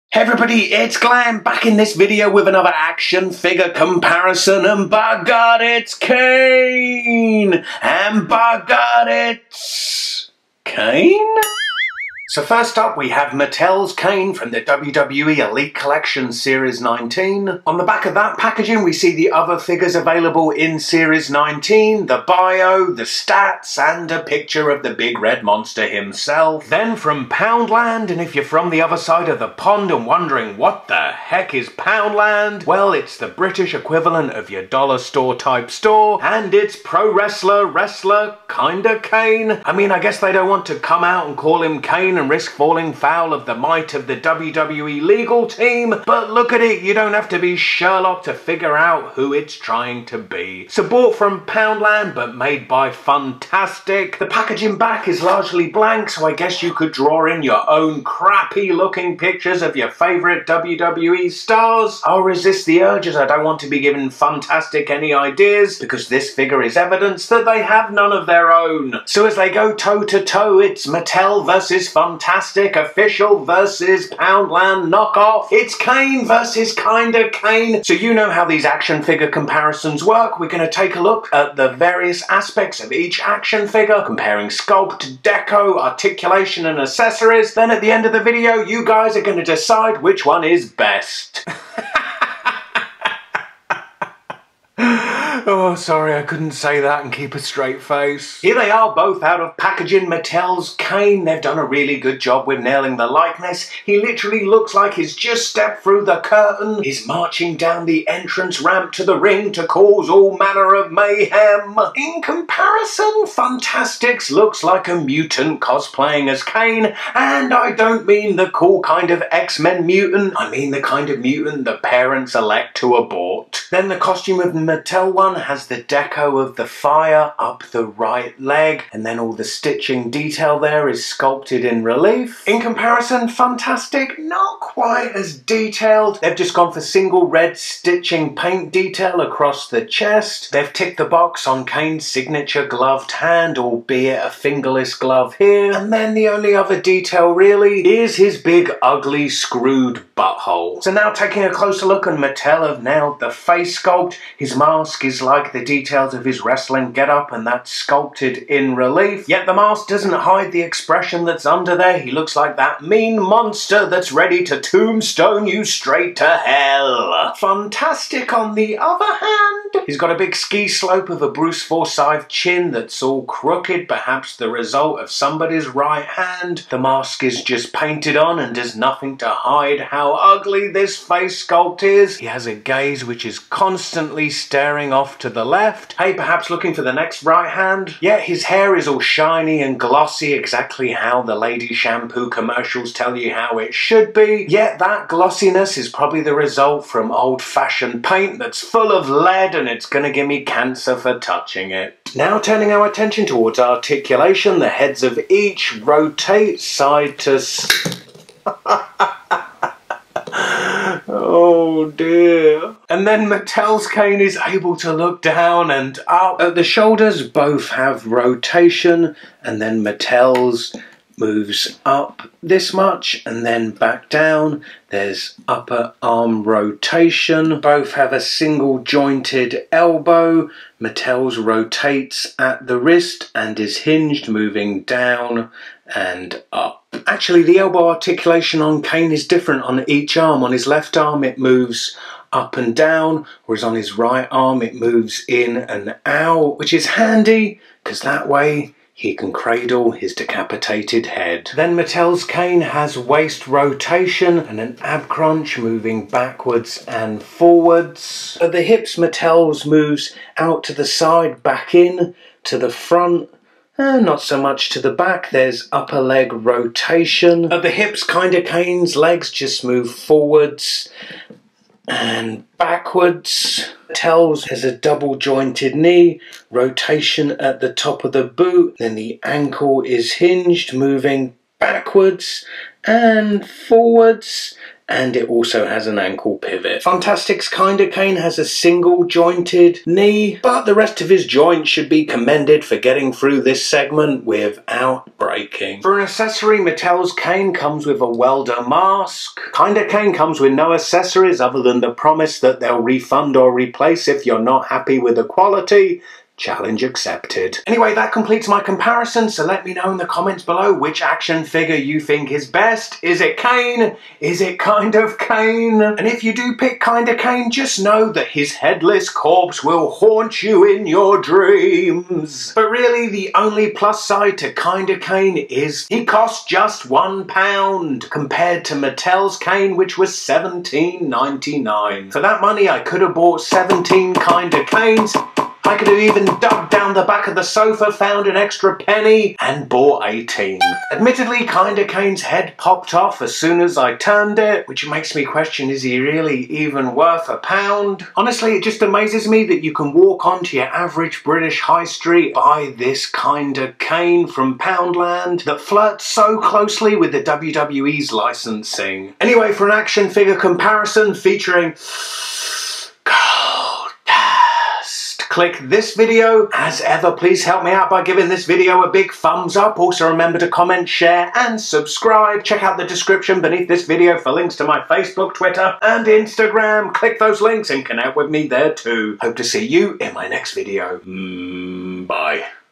Mattel vs Poundland KANE WWE Action Figure Comparison
Hey everybody it's an Action Figure Comparison as the WWE Elite Collection Series 19 KANE faces off against his Poundland counterpart!